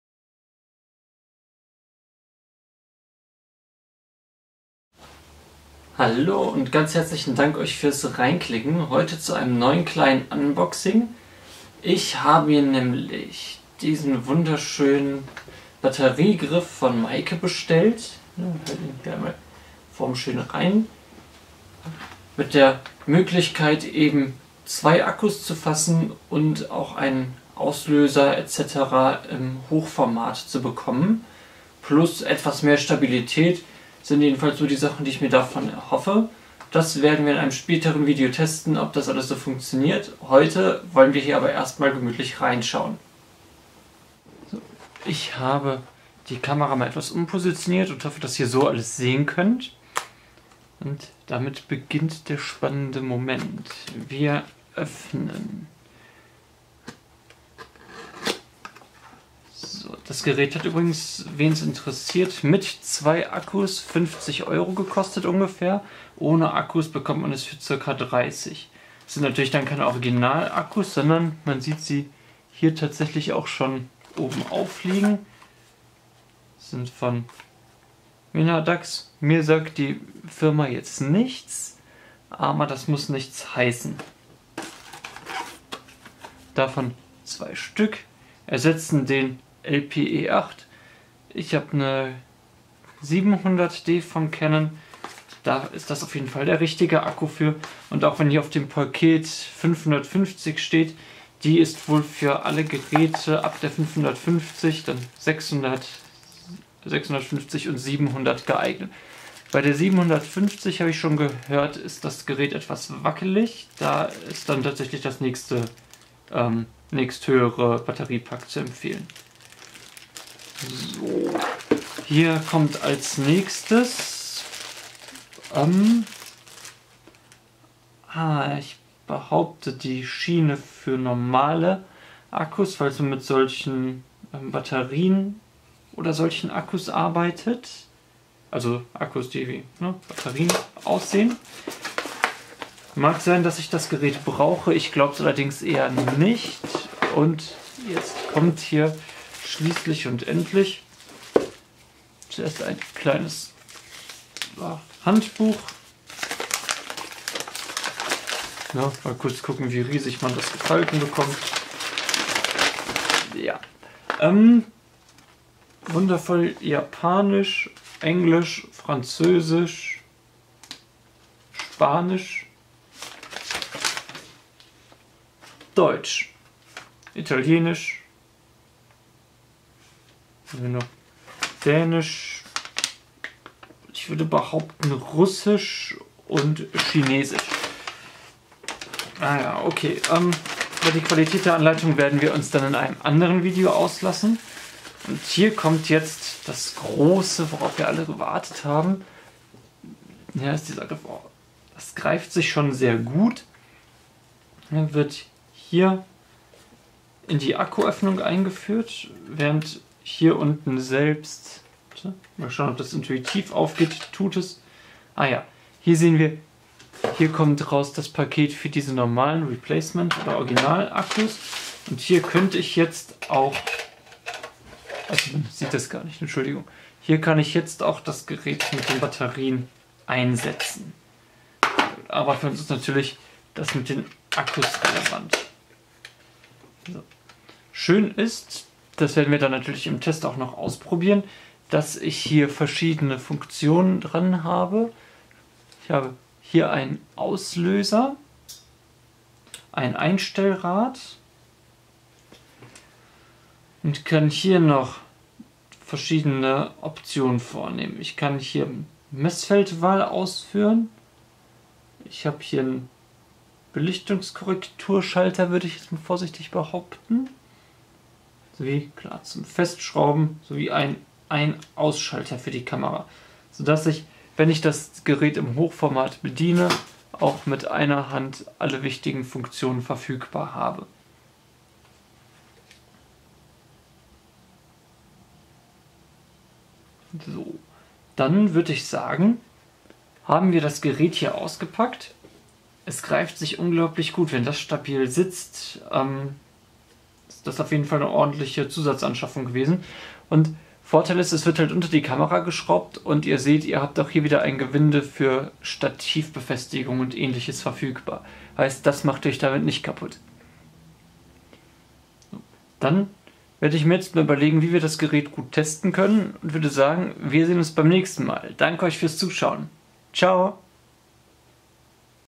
[Unboxing] Meike Batteriegriff MK-550D
UPDATE
Leider kann ich kein Testvideo machen, da bereits nach zwei Tagen ein schwerer Defekt am Griff auftrat und ich ihn somit zurückschicken musste. Ich schätze, ich habe ein Montagsprodukt erwischt. Da mein Fehler jedoch laut Rezensionen häufiger auftritt, ahbe ich von einer Neubestellung abgesehen und stattdessen einen anderen Griff gekauft. Da ich den Griff aber nicht vollumfänglich ausprobieren konnte, entfällt der Test des Batteriegriffs. Sorry!

Kurz nach der Bestellung kam auch schon mein Meike Batteriegriff an. In diesem Video könnt ihr beim auspacken dabei sein :-)

Der Batteriegriff in meinem Unboxing ist für die Canon-Kameras EOS 550D bis 700D geeignet.